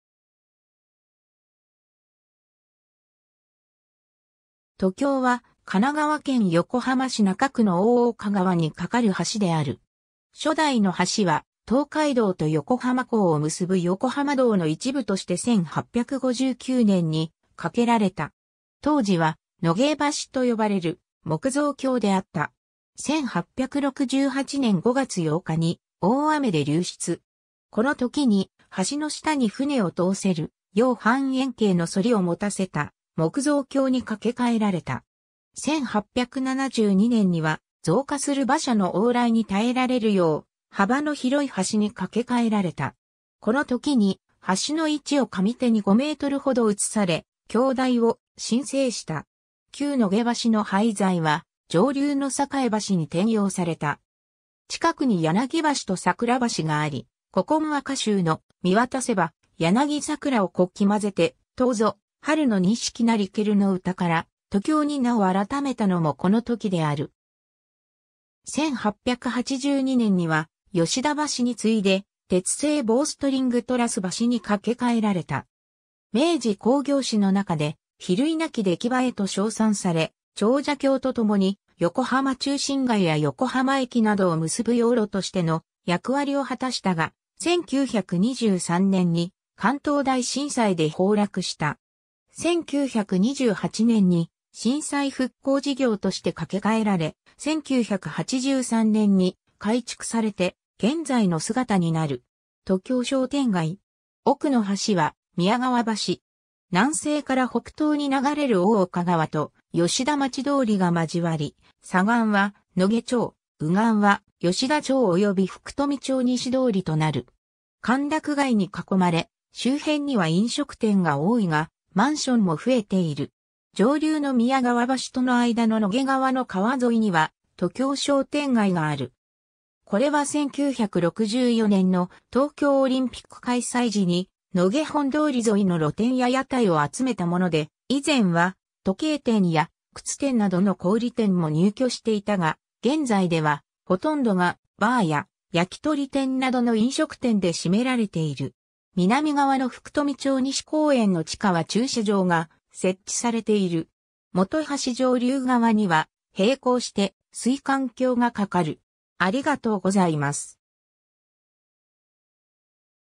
都橋
都橋（みやこばし）は、神奈川県横浜市中区の大岡川に架かる橋である。
 初代の橋は、東海道と横浜港を結ぶ横浜道の一部として1859年（安政6年）に架けられた。
当時は「野毛橋」と呼ばれる木造橋であった。
1868年（明治元年）5月8日に大雨で流失。
この時に、橋の下に船を通せるよう半円形の反りを持たせた木造橋に架け替えられた。
1872年には、増加する馬車の往来に耐えられるよう、幅の広い橋に架け替えられた。
この時に橋の位置を上手に5mほど移され、橋台を新製した。
旧野毛橋の廃材は、上流の栄橋（黄金町駅近く）に転用された。
近くに柳橋と桜橋があり、古今和歌集の「見渡せば柳桜をこきまぜて都ぞ春の錦なりける」の歌から「都橋」に名を改めたのもこの時である。
1882年（明治15年）には吉田橋に次いで鉄製ボーストリングトラス橋に架け替えられた。
『明治工業史』の中で「比類なき出来栄え」と称賛され、長者橋とともに横浜中心街や横浜駅（現在の桜木町駅）などを結ぶ要路としての役割を果たしたが、1923年（大正12年）に関東大震災で崩落した。
1928年（昭和3年）に震災復興事業として架け替えられ、1983年（昭和58年）に改築されて、現在の姿になる。
 都橋商店街。
奥の橋は宮川橋。
 南西から北東に流れる大岡川と、吉田町通り（横浜道）が交わり、左岸（北西側）は野毛町、右岸は吉田町および福富町西通となる。
歓楽街に囲まれ、周辺には飲食店が多いがマンションも増えている。
上流の宮川橋との間の野毛側の川沿いには都橋商店街がある。
これは1964年の東京オリンピック開催時に野毛本通り沿いの露店や屋台を集めたもので、以前は時計店や靴店などの小売店も入居していたが、現在ではほとんどがバーや焼き鳥店などの飲食店で占められている。
南側の福富町西公園の地下は駐車場が設置されている。
本橋上流側には並行して水管橋が架かる。